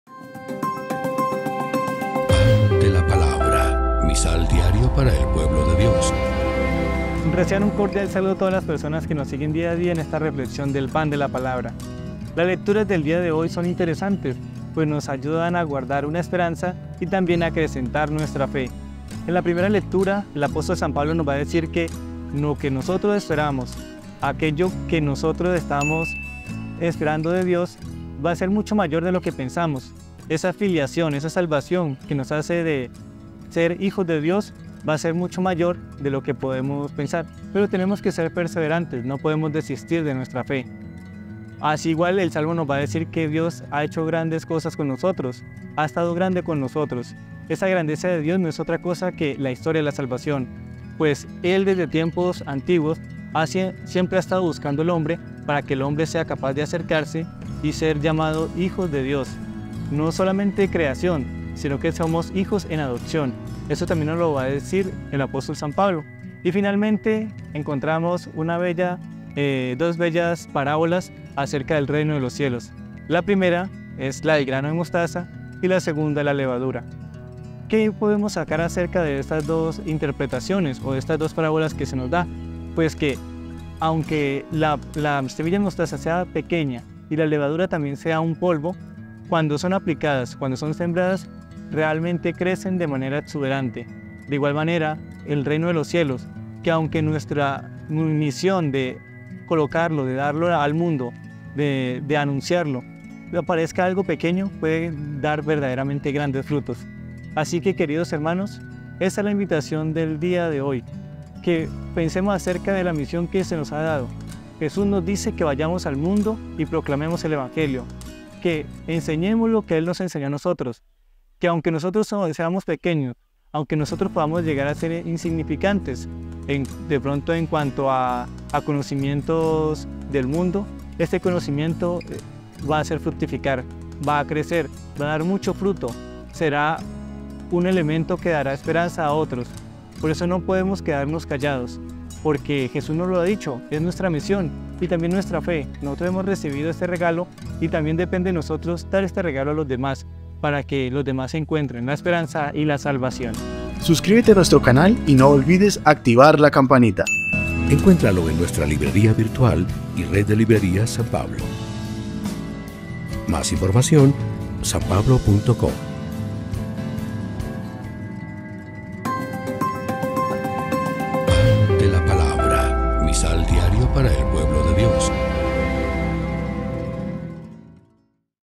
EVANGELIO 30 DE OCTUBRE DE 2023 | Pan de la Palabra
Esta es la Reflexión Evangelio del Lunes 30 de octubre de 2023
📜 Lecturas del día (Rm 8, 12-17; Salmo 67; Lc  13, 10-17).

🙏En el día de domingo, además de participar en la celebración eucarística, que ciertamente es el punto culminante de la jornada, ¿ayudamos a enderezarse a las personas que están agobiadas por diversos males?

🤲Señor Jesús, que dijiste “la mies es mucha y los obreros pocos”, queremos ser apóstoles de tu amor, para llevar un mensaje de alegría y esperanza a los hermanos.
Amén

Encuéntralo en nuestra librería virtual, en nuestra red de librerías o mercado libre.

📲 Disponible en eBook:
OCTUBRE DE 2023:

Hno. Oscar Huepe, religioso de la Sociedad de San Pablo.